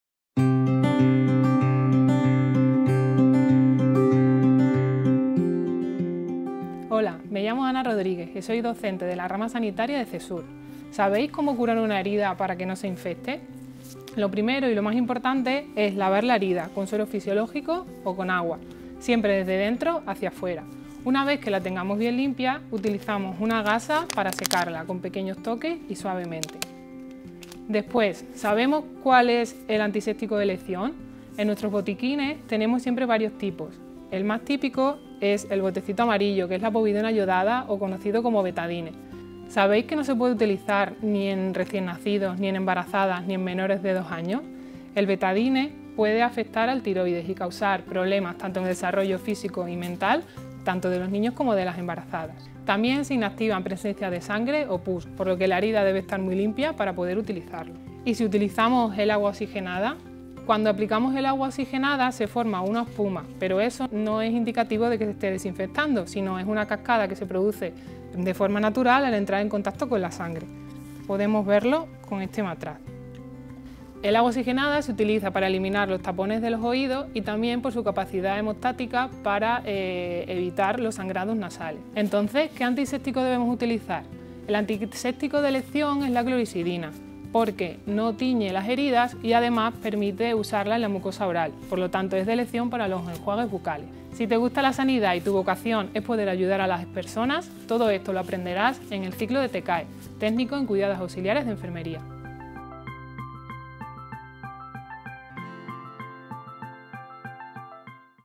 🤕 ¿Sabes curar una herida? | FP TCAE
¡Descubre los pasos esenciales para curar heridas de manera rápida y efectiva! En este video, nuestra docente Ana Rodríguez los explica para que podamos llevar a cabo la cura correctamente. ¡No te pierdas estos consejos para una recuperación exitosa!

¿Quieres unirte a la FP de Cesur?
🚨 Llámanos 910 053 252 y matricúlate 🚨